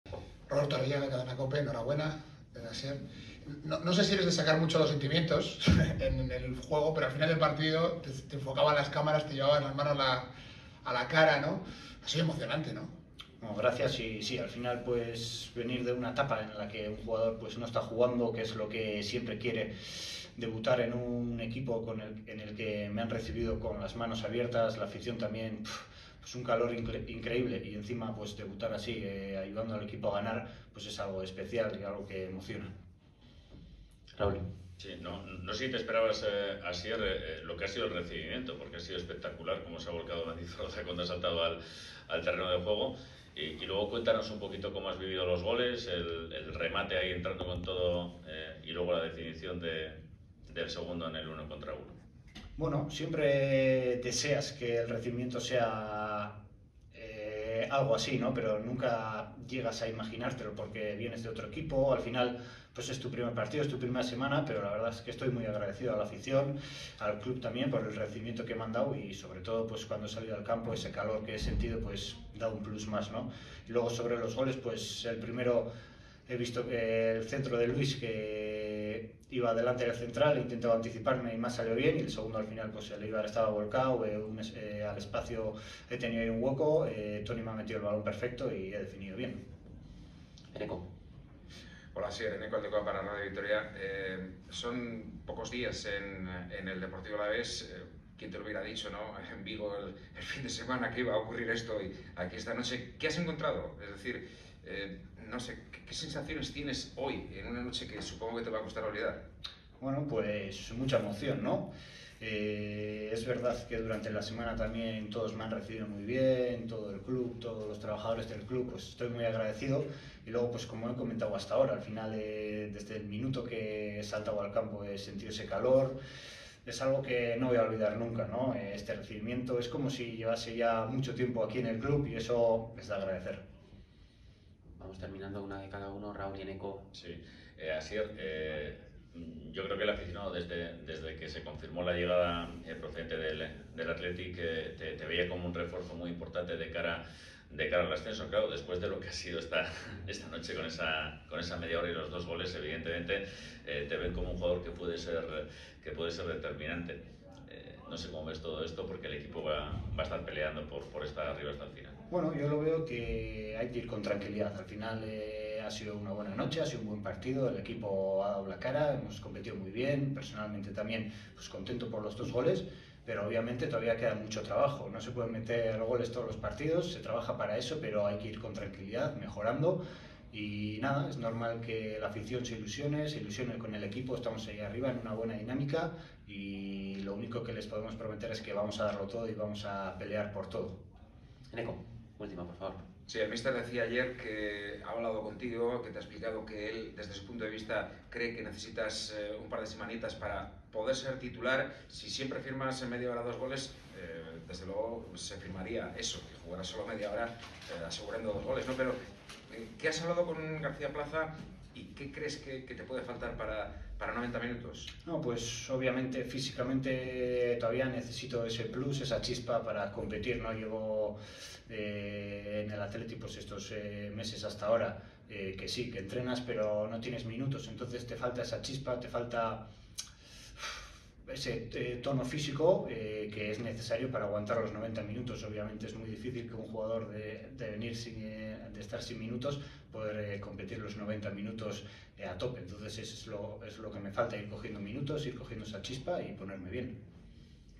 Asier Villalibre: "Debutar así es algo especial; algo que emociona" | Deportivo Alavés
Declaraciones de Asier Villalibre tras el partido correspondiente a la Jornada 26 de LaLiga SmartBank, Deportivo Alavés vs. SD Eibar